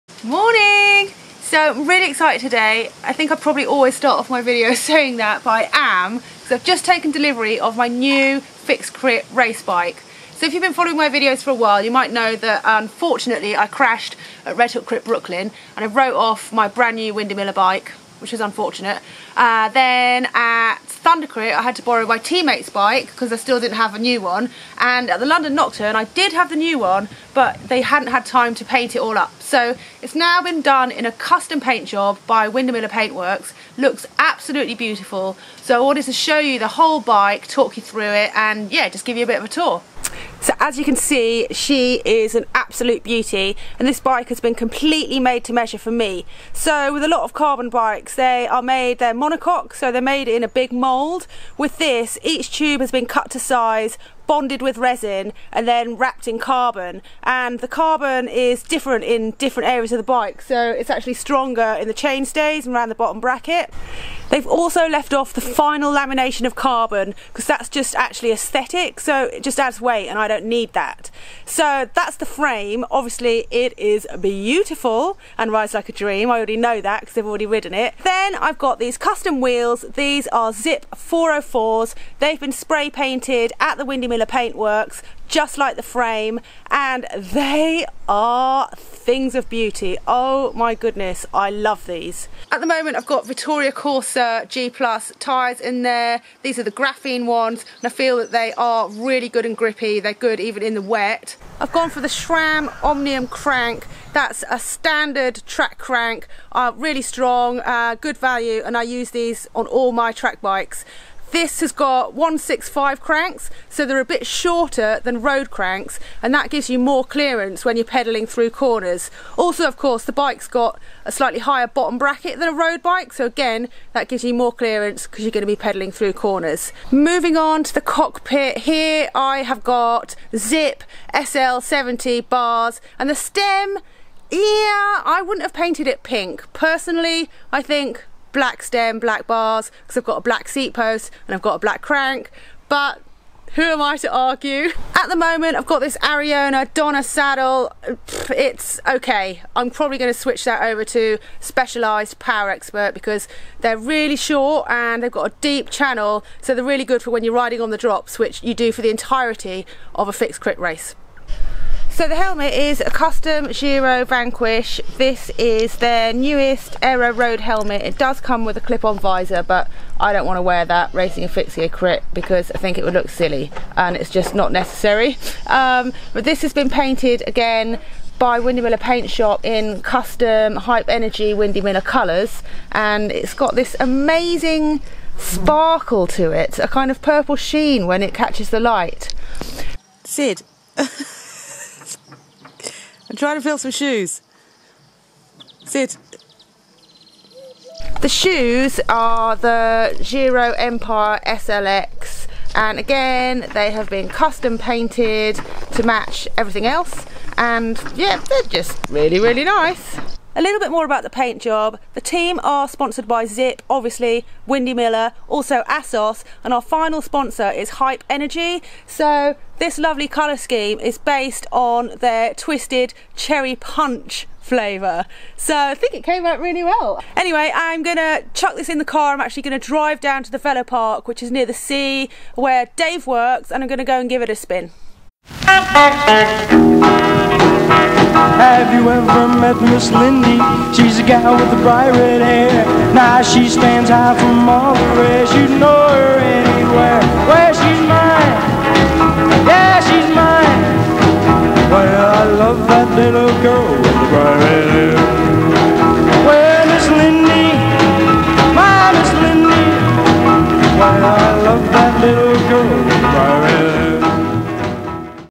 Possibly the best bike in the world?
Not only does my new custom carbon fixed gear crit race bike feel like a dream, it looks like one too. Check out my custom hand built bike, custom Giro Vanquish helmet and custom Giro Empire SLX shoes.

NEW VIDEOS (usually) EVERY FRIDAY